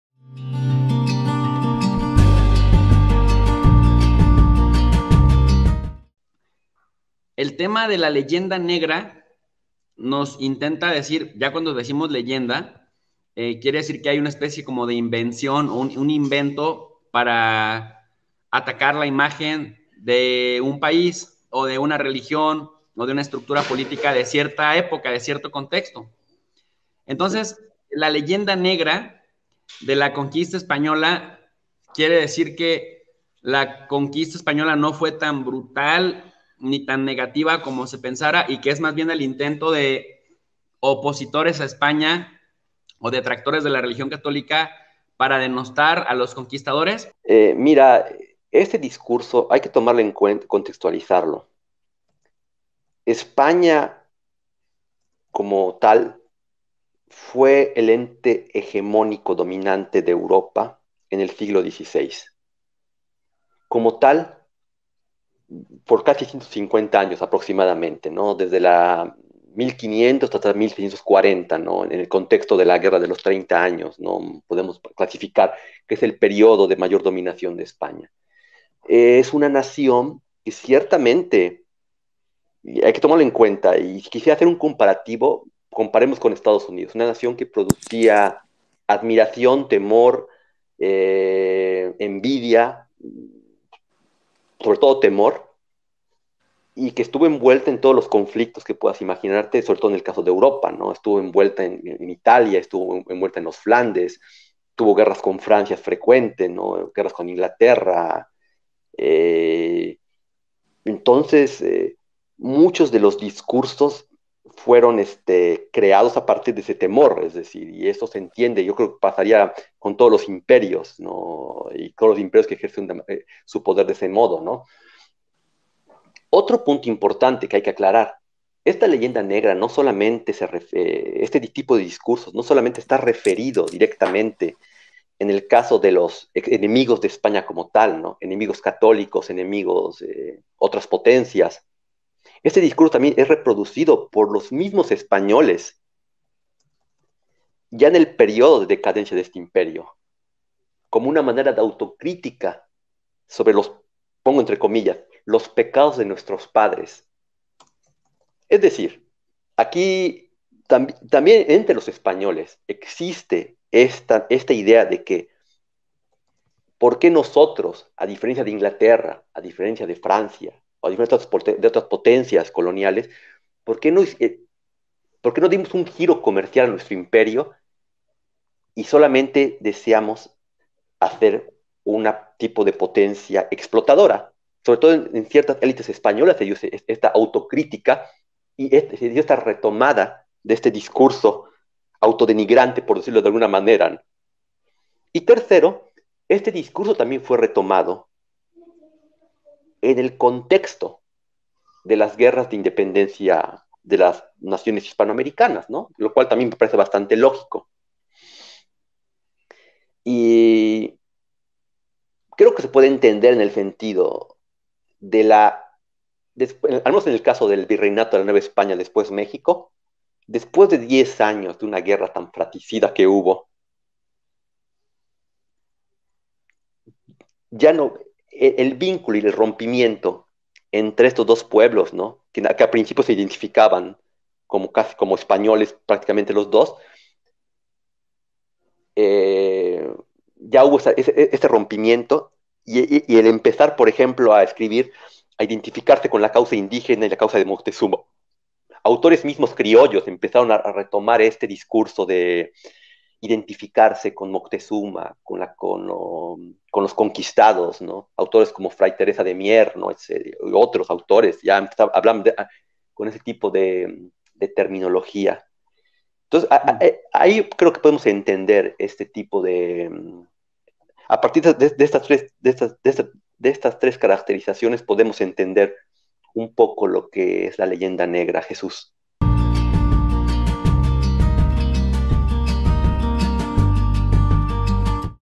LA LEYENDA NEGRA ESPAÑOLA
En este video el Dr. Manuel Méndez Alonzo, nos comparte la definición del término "La leyenda negra española".